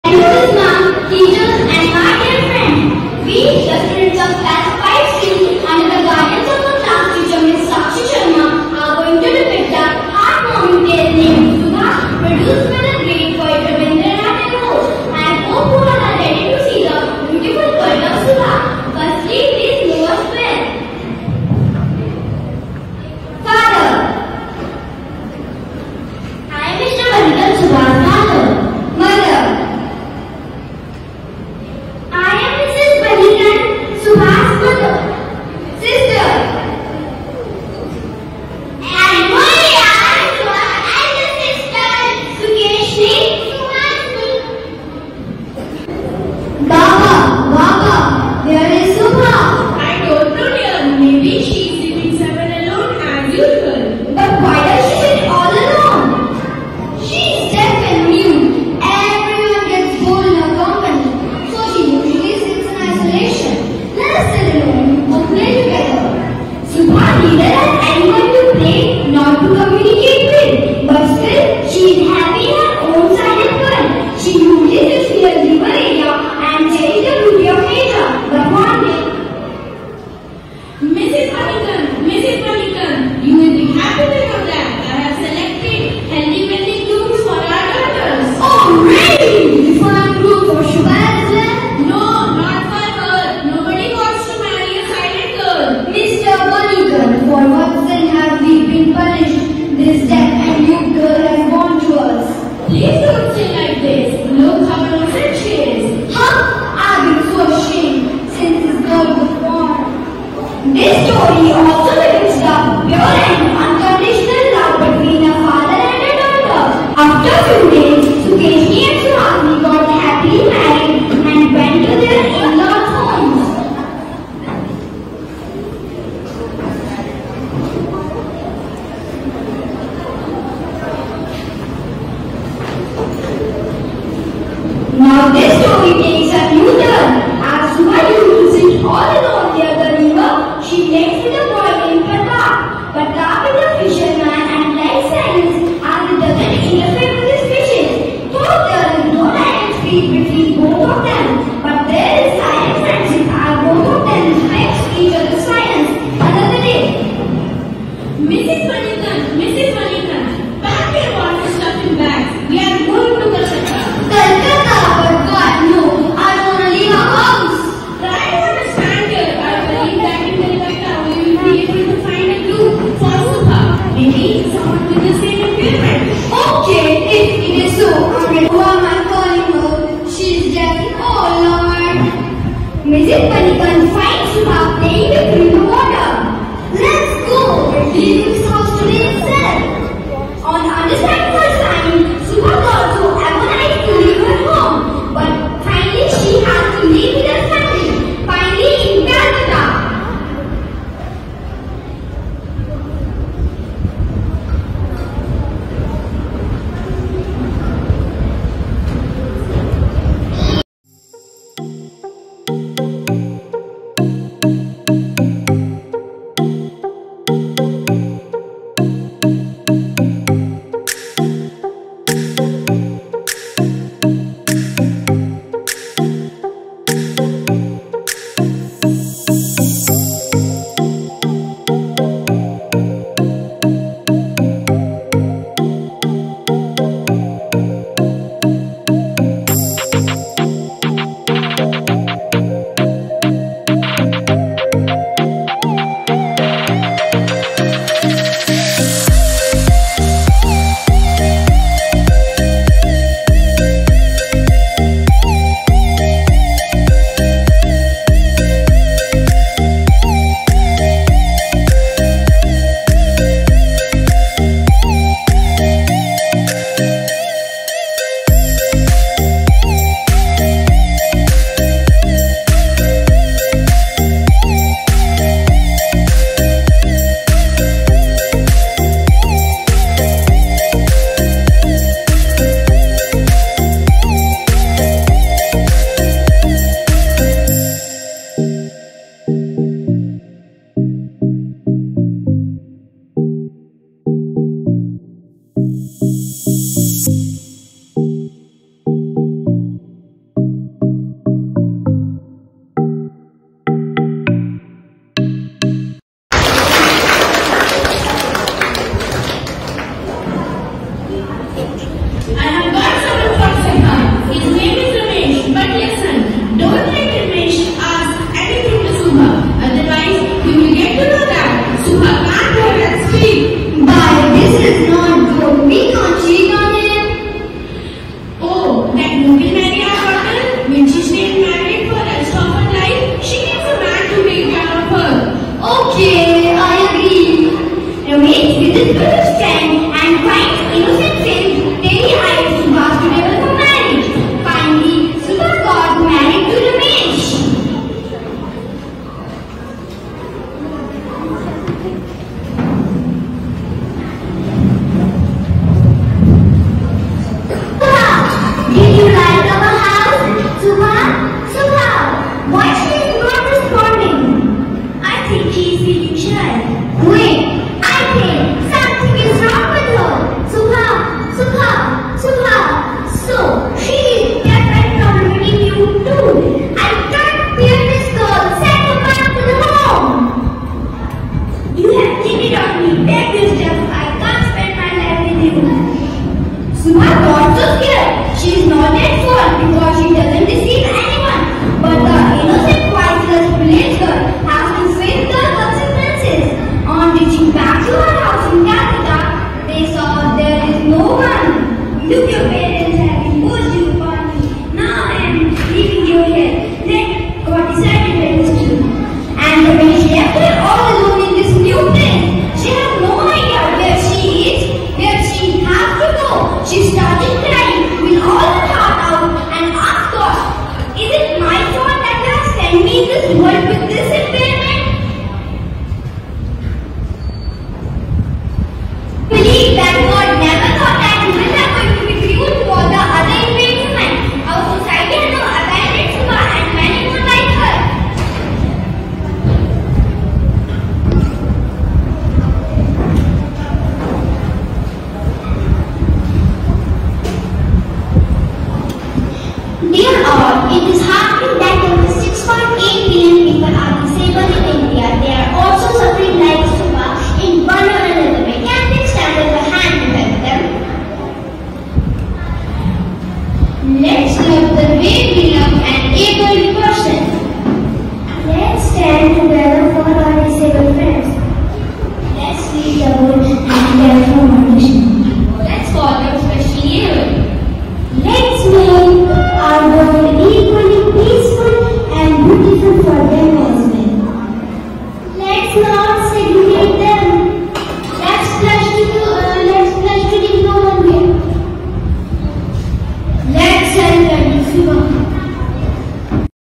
Subha: by Rabindranath Tagore || Full play script
SUBHA FULL SCRIPT

Narrator: Good morning respected chief guest, chairman sir, principal mam, headmistress mam and amazing audience.
We, the students of Class 5C under the guidance of our class teacher Ms. Sakshi Sharma are going to depict a heartwarming tale named SUBHA produced by the great poet Rabindranath Tagore.
I hope you all are ready to see the beautiful world of Subha..
Firstly, please know us well.
Father: I’m Mr. Banikanth, Subha’s father
Mother: I’m Mrs. Banikanth, Subha’s mother
And we’re Subha’s elder sisters, Sukeshani, Suhasini.

Sukeshani: Baba! Baba! Where is subha?
Father: I don’t know dear. Maybe she is sitting somewhere alone! As usual.
Suhasini: But why does she sit all alone?
Mother: She is deaf and dumb, everyone gets bored in her company, so she usually sits in isolation. Let her sit alone, you both play together.
Narrator: Subha neither has anyone to play nor to communicate with. But still, she is happy in her own silent world. She usually sits near river area, and cherish the beauty of nature. But one day…
Father: Mrs. Banikanth! You’ll be happy to know that I have selected healthy wealthy grooms for our daughters.
Mother: Oh Really! Did you find groom for Subha as well?
Father: No! not for her. Nobody wants to marry a silent girl.
Mother: Mr. Banikanth, for what sin have we been punished, this deaf and dumb girl has born to us.
Father: Please don’t say like this. Look how innocent she is..
Mother: Huh! I’ve been so ashamed, since this girl was born.
Narrator: This story also depicts the pure and unconditional love between a father and a daughter. After few days, Sukeshani and Suhasini got happily married and went to their in-law’s homes.  Now this story takes a new turn, As Subha used to sit all alone near the river, she met with a boy named Partap.  Partap is a fisherman, and likes silence as it does not interfere with his fishes. Though, there is no direct speech between both of them but there is silent friendship as both of them respects each other’s silence. Another day…
Father: Mrs. Banikanth, pack the important stuff in bags, we’re moving to Calcutta.
Mother: Calcutta! But why? No, I don’t want to leave our house.
Father: Try to understand dear, I believe that in Calcutta we’ll be able to find a groom for Subha. Maybe someone with the same impairment.
Mother: Okay! If it is so, I am ready to move.
Mother: Subha! Subha! Oho why am I calling her, she is deaf. Oh lord.
Narrator: Mrs. Banikanth finds Subha playing with river water.
Mother: Let’s go, we are leaving this house today itself.
Narrator: On understanding her signs, subha got so agonized to leave her home. But finally has to leave with her family.
Finally in Calcutta...
Father: I’ve got someone for Subha. His name is Ramesh. But listen, don’t let Ramesh ask anything to Subha otherwise he will get to know that subha can’t hear and speak.
Mother: By this is not good, we can’t cheat on him.
Father: Oho! Then who will marry our daughter? Will she stay unmarried for the rest of her life? She needs a man to take care of her.
Mother: Okay! I agree.
Narrator: Ramesh visits with his friend and finds innocent faced, teary-eyed Subha suitable for marriage. Finally, Subha got married to Ramesh.
Mother-in-law: Subha! Did you like our house? Subha! Subha!
Why is she not responding?
Ramesh: I think she is feeling shy.
Mother-in-law: Wait! I think, something is wrong with her.
Subha! Subhaa! Subhaaaa!
So, she is deaf and probably dumb too.
I can’t bear this girl, send her back to her home.
Ramesh: You have cheated on me. Pack your stuff, I can’t spend my life with you.
Narrator: Subha got so scared. She is not at fault because she doesn’t deceive anyone. But the innocent voiceless village girl has to face the repercussions.
On reaching back to her house in Calcutta, they saw there is no one.
Ramesh: Look your parents have imposed you upon me. Now I am leaving you here. Let God decide your destiny.
Narrator: And Ramesh left her all alone in this new place. She has no idea where she is, where she has to go. She started crying will all her heart-out and asks God….
Narrator: Subha: Is it my fault that you’ve sent me in this world with this impairment….?
Narrator: But we believe that God never thought that humans are going to be cruel towards the other impaired human. Our society has now abandoned Subha and many more like her.
Narrator 2: Dear all, it’s disheartening that 26.8 million people are disabled in India. They are also suffering like Subha, in one or another way. Can we extend our hand to help them?
Let’s love them the way we love an abled person.
Let’s stand up together for our Disabled Friends.
Let’s delete the word handicapped from our dictionary.
Let’s call them Specially Abled.
Let’s make our world equally peaceful and beautiful for them as well.
Let’s not stigmatize them.
Let’s pledge to, “Leave No One Behind”.
Let’s help every Subha…

Script written by: Ms. Sakshi Sharma